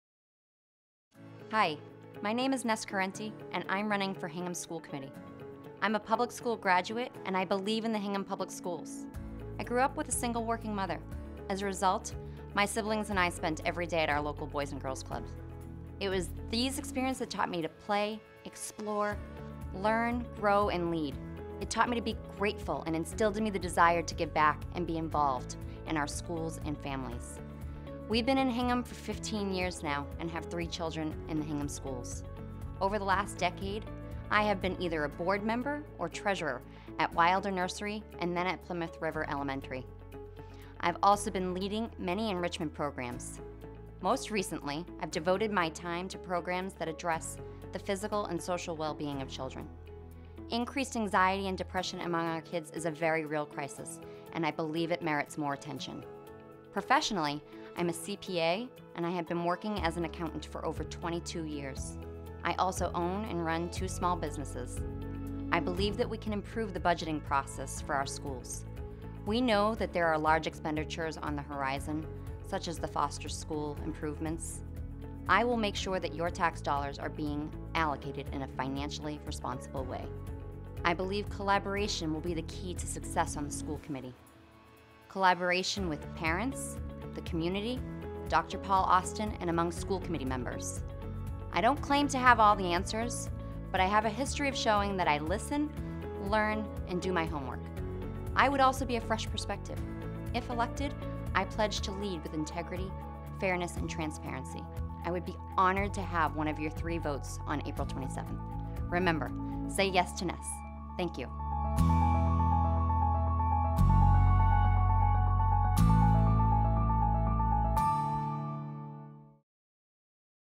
Say Yes to Nes!
Nes Correnti is running for Hingham School Committee!